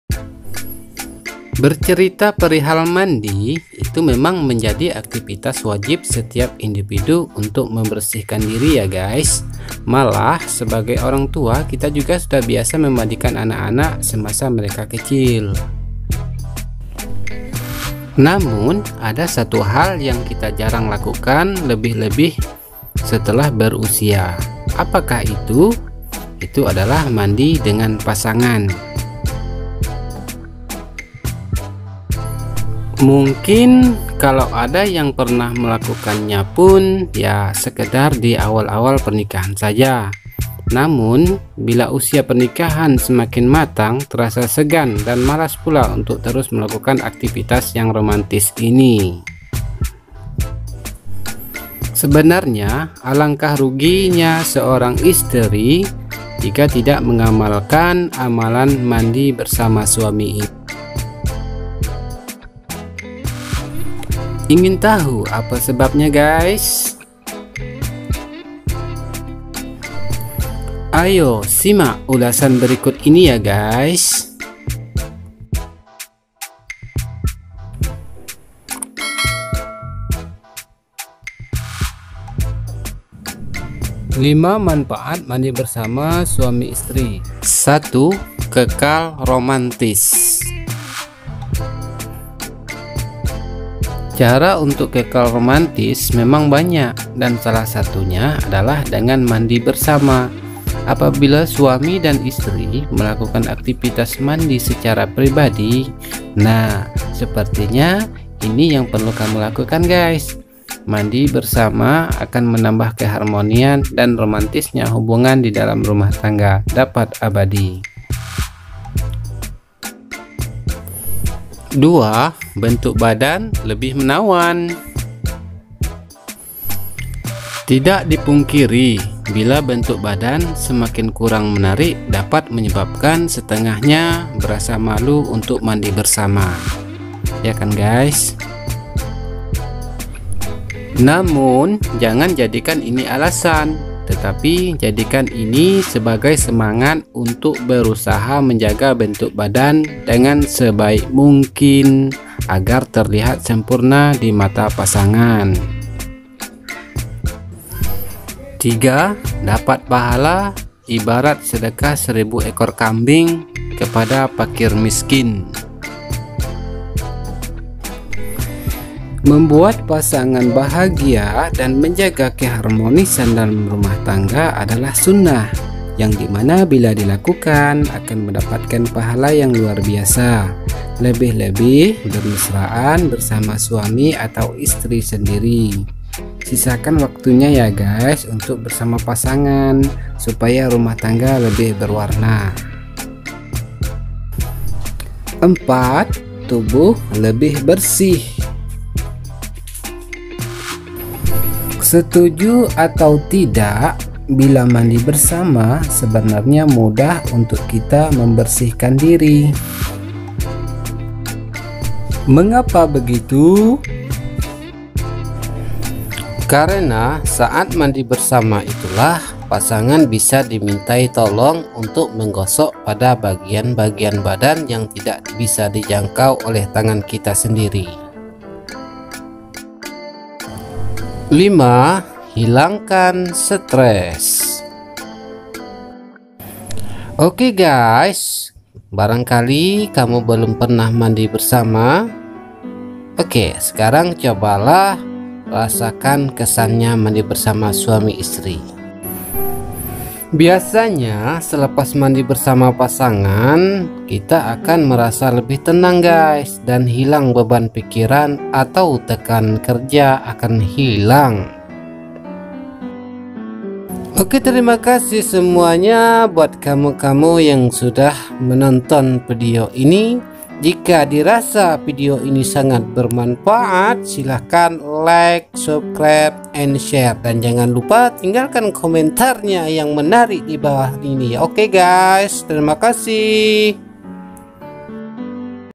hukum mandi bersama suami istri menurut Islam
hukum mandi bersama suami istri menurut Islam, Tak melulu dengan begituan, keharmonisan pasangan suami istri rupanya bisa diwujudkan dengan cara-cara sederhana, misalnya dengan mandi bersama.

Meskipun terkesan aneh dan tak biasa, rupanya kebiasaan ini justru bisa merekatkan hubungan.

simak video ini sampai habis biar gak gagal paham..